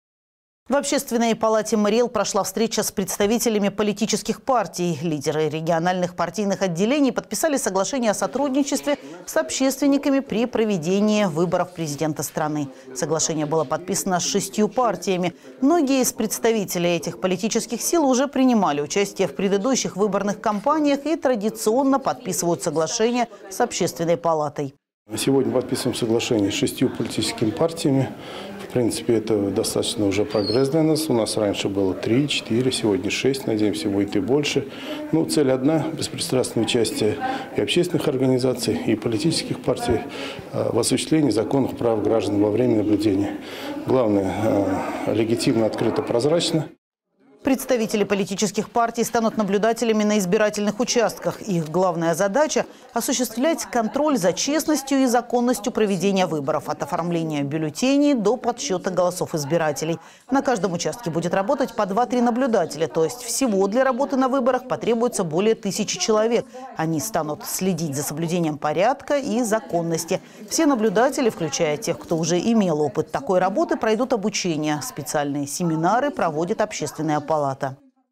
В общественной палате Марий Эл прошла встреча с представителями политических партий
Лидеры региональных партийных отделений подписали соглашение о сотрудничестве с общественниками при проведении выборов президента страны. Соглашение было подписано с шестью партиями. Многие из представителей этих политических сил уже принимали участие в предыдущих выборных кампаниях и традиционно подписывают соглашение с Общественной палатой.